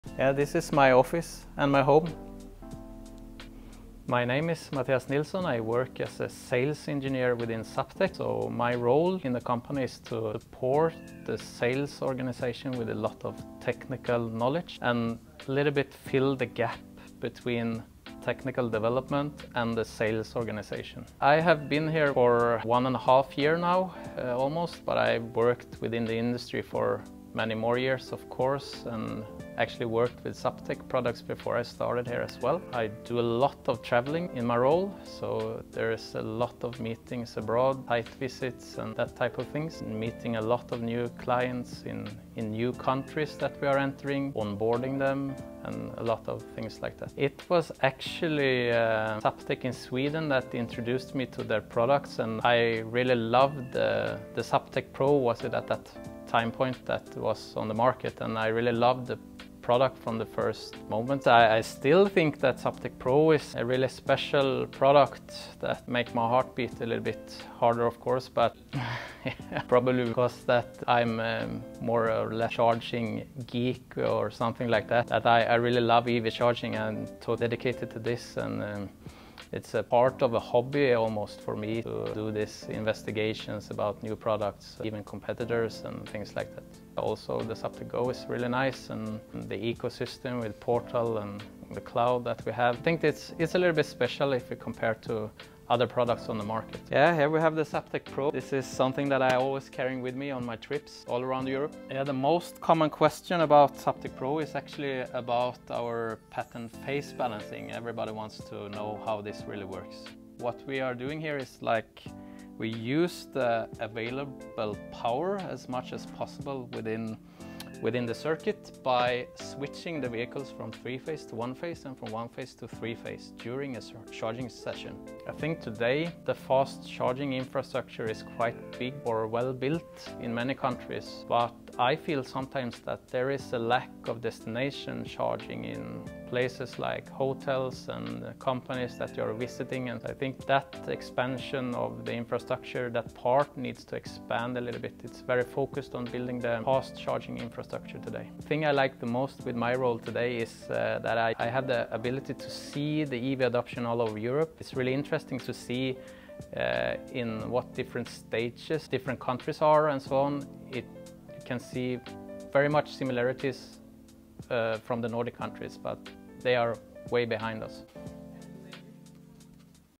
Meet Mattias Nilsson, Sales Engineer
Our products make his heart beat a little harder—that's dedication! 🙌🏽 Meet Mattias Nilsson, our Sales Engineer at Zaptec. He is bridging the gap between tech innovation and sales in our company. Always on the move, Mattias brings our solutions worldwide, forging connections and paving the way for new markets 🚀